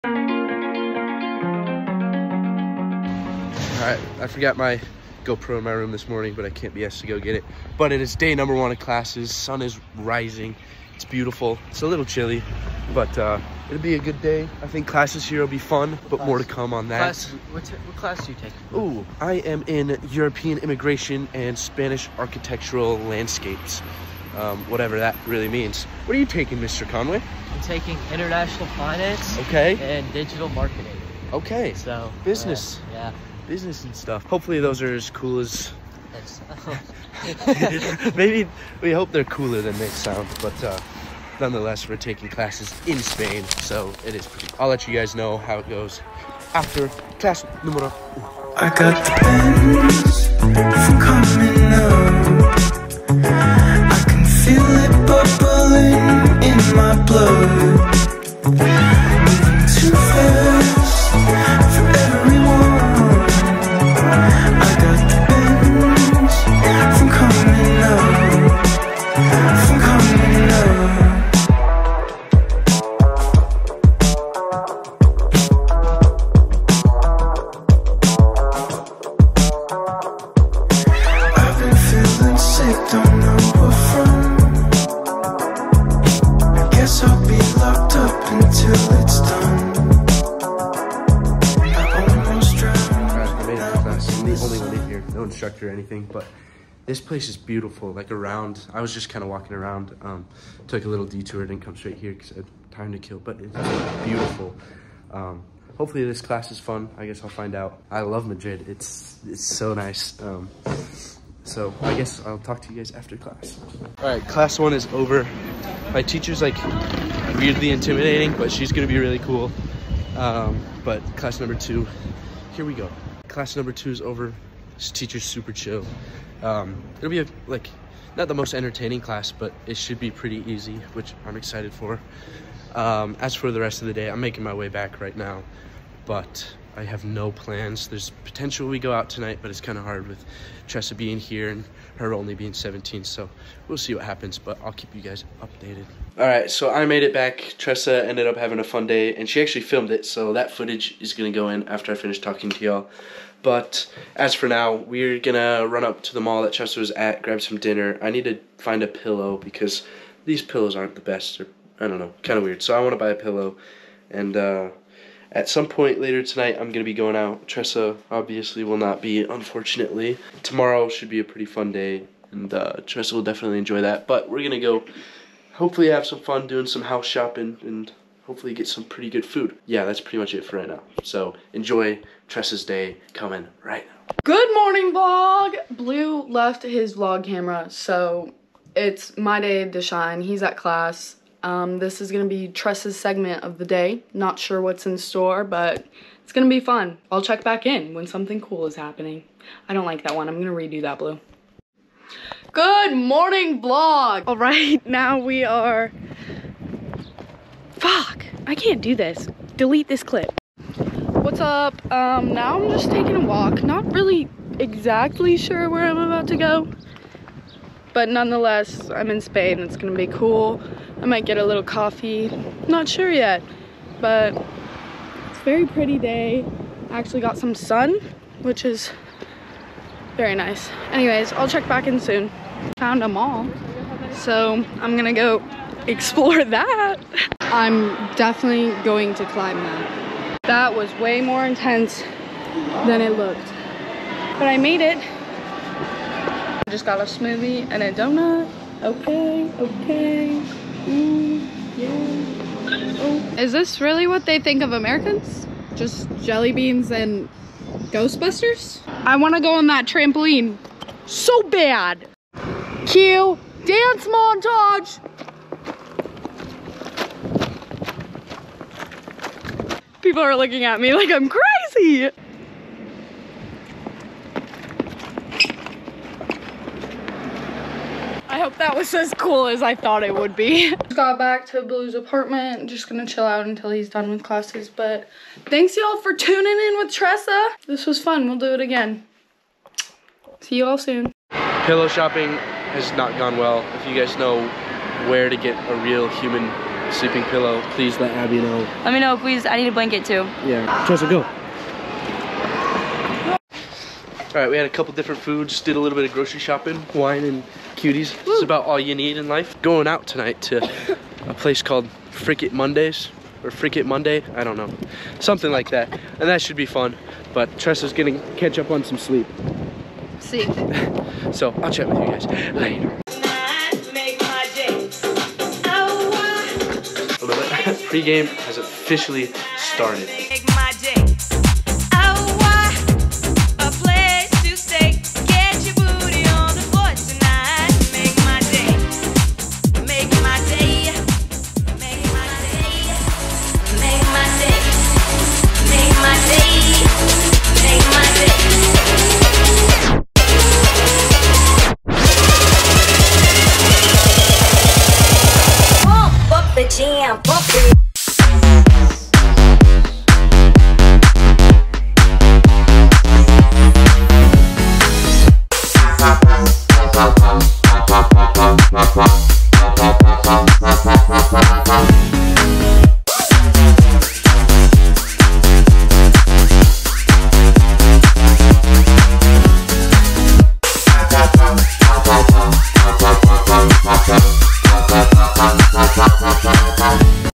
VLOG #91: First day of class in a different country
Today was my first day of classes in the beautiful city of Madrid, Spain. I forgot my camera at my place so I did a phone vlog while my sister hijacked the GoPro and recorded a little bit of her day, which was goofy nonetheless. After getting through some new intimidating teachers and meeting some new people I celebrated by spending countless hours looking for a pillow. WHY DOESNT SPAIN HAVE NORMAL SLEEPING PILLOWS PLEASE HELP. After an unsuccessful search I cheered myself up with some old rock/techno music type music and went to one of the most famous clubs here in Madrid call, well, I cant really say it but it rhymes with lucking, and is called ing mondays or something like that, and it was an absolute blast. Thanks for tuning in and I will see ya'll tmrw!